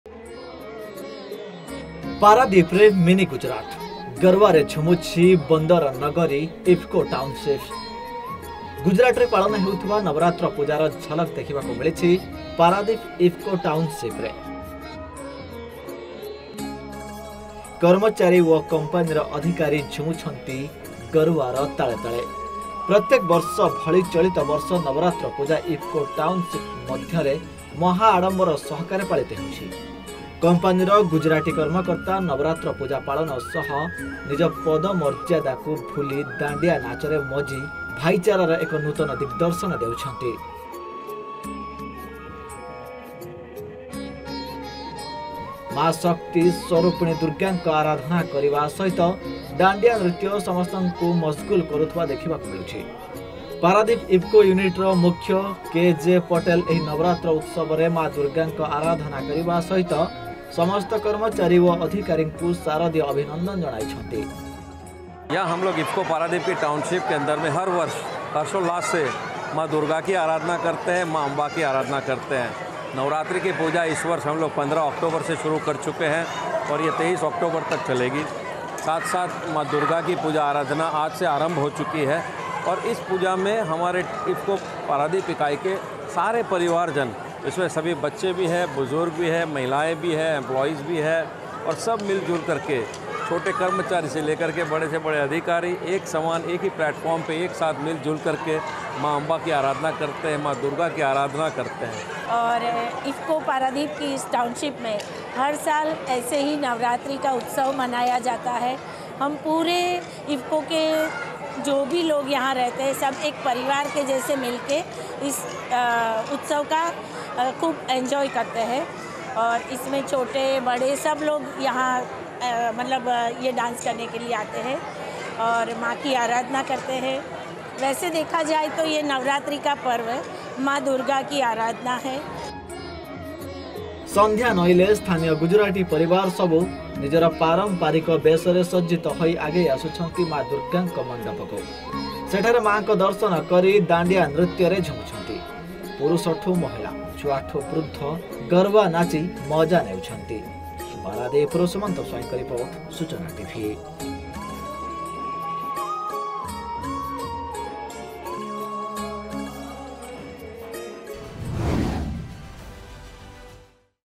ଇଫକୋ ଟାଉନସିପ ଝୁମୁଛି ଦାଣ୍ଡିଆର ତାଳେ ତାଳେ
ପାରାଦୀପରେ ମିନି ଗୁଜୁରାଟ। ଗରୱାରେ ଝୁମୁଛି ବନ୍ଦର ନଗରୀର ଇଫକୋ ଟାଉନସିପ।  ଗୁଜୁରାଟରେ ପାଳନ ହେଉଥିବା ନବରାତ୍ର ପୂଜାର ଝଲକ ଦେଖିବାକୁ ମିଳିଛି ପାରାଦୀପ ଇଫକୋ ଟାଉନସିପରେ। କର୍ମଚାରୀ ଓ କମ୍ପାନୀର ଅଧିକାରୀ ଝୁମୁଛନ୍ତି ଗରୱାର ତାଳେ ତାଳେ। ପ୍ରତ୍ୟେକ ବର୍ଷ ଭଳି ଚଳିତ ବର୍ଷ ନବରାତ୍ର ପୂଜା ଇଫକୋ ଟାଉନସିପ ମଧ୍ୟରେ ମହା ଆଡମ୍ବର ସହକାରେ ପାଳିତ ହେଉଛି।

Dear Viewers,